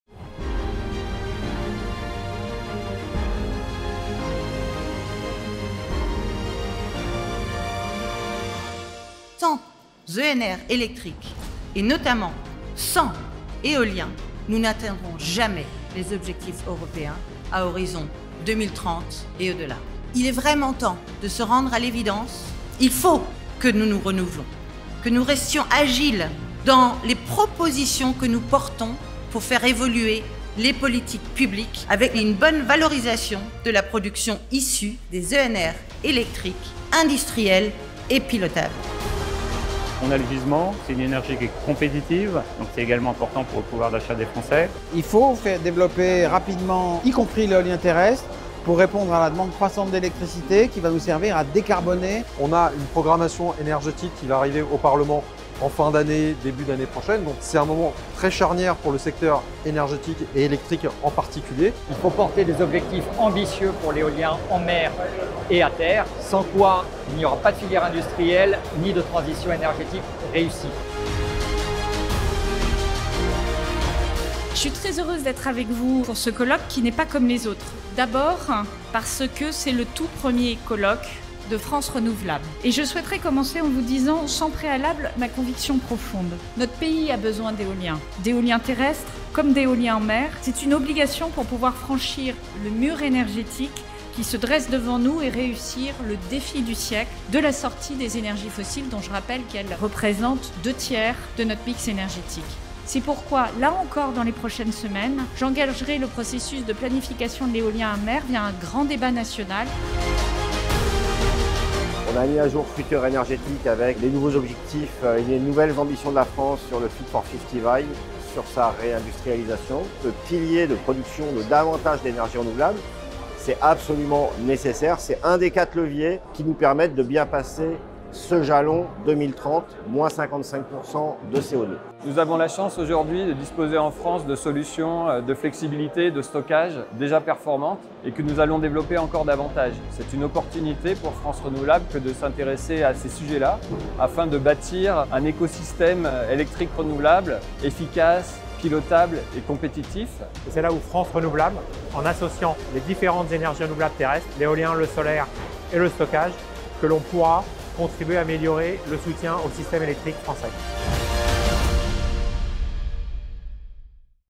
Revivez le 13ème Colloque National Eolien du 4 et 5 octobre 2023
Le Colloque National Éolien 2023 a marqué la 13ème édition de l’évènement majeur de l’éolien en France et la 1ère édition sous les couleurs de France Renouvelables. Cette année plus de 3000 visiteurs sont venus de toute la France et d’Europe pour faire avancer la transition énergétique en France.

Depuis près de 25 ans, la filière éolienne œuvre à la transition énergétique de notre pays.